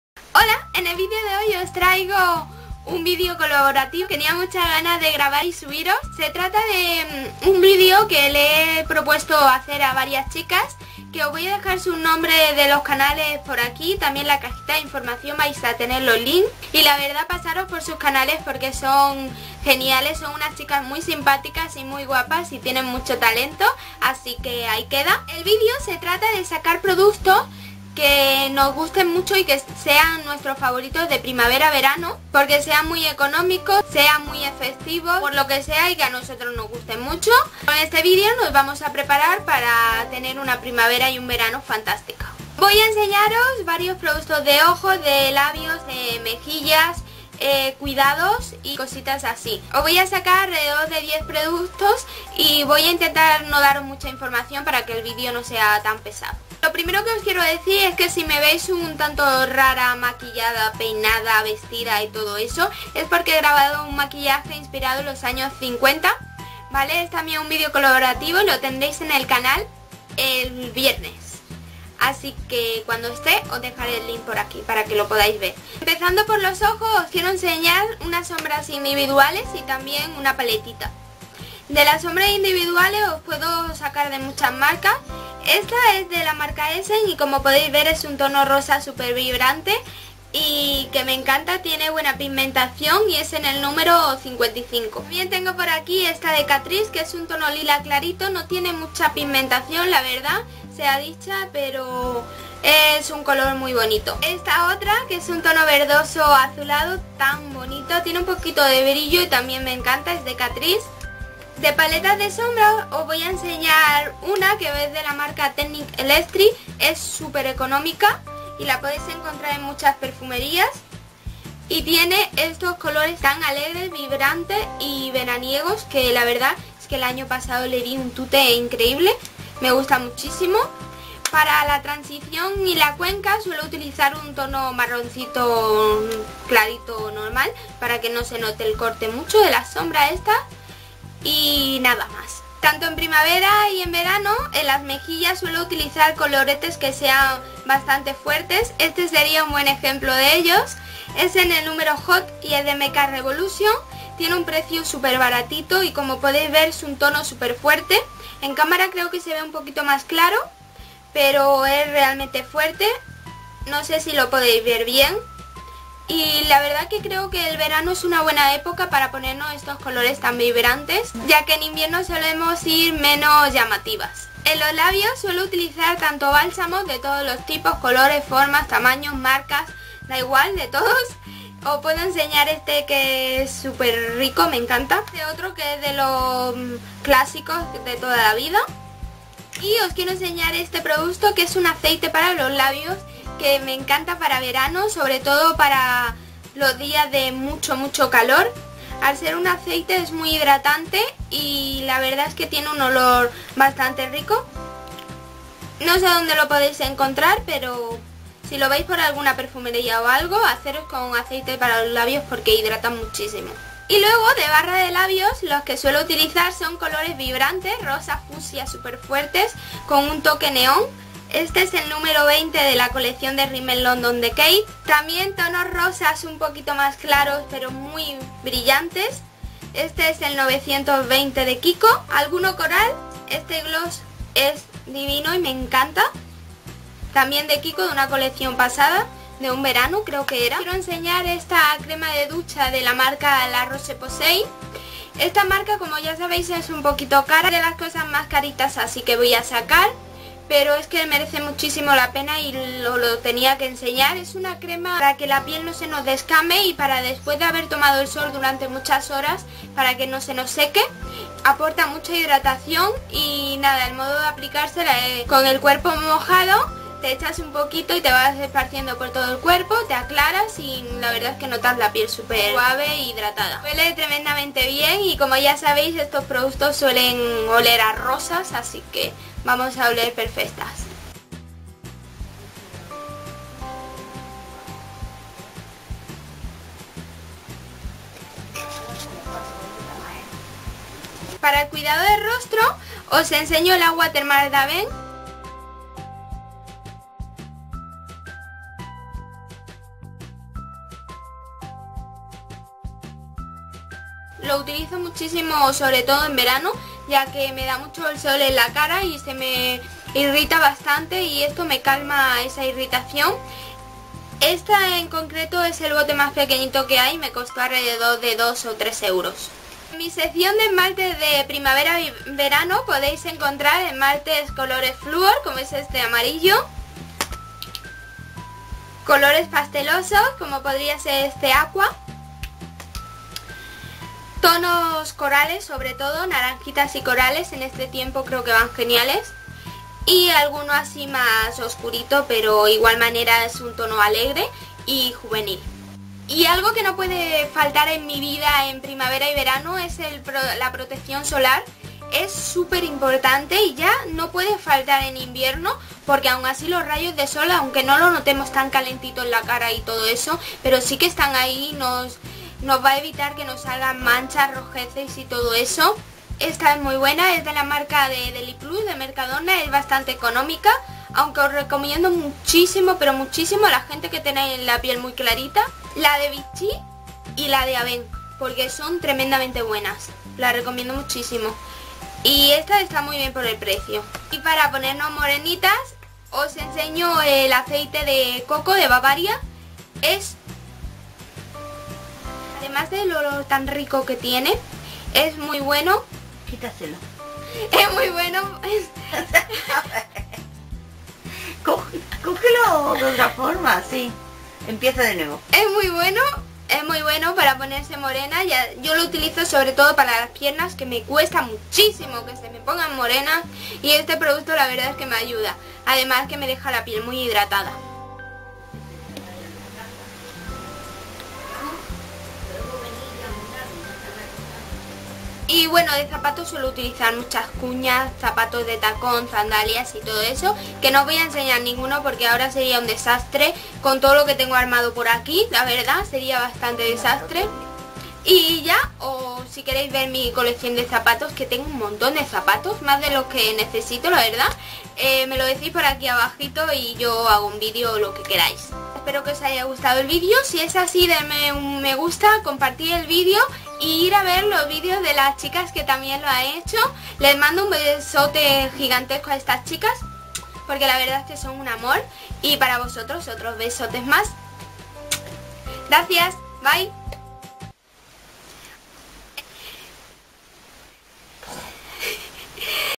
Favoritos para Primavera y Verano ♥ ¿Nos preparamos para Verano?
Hola! El video de hoy es un colaborativo que le he propuesto hacer a unas chicas preciosas!! Es un video donde vamos a sacar productos de todo tipo que sean nuestros favoritos para Primavera-Verano y así podemos estar preparadas para disfrutar al máximo este tiempo!!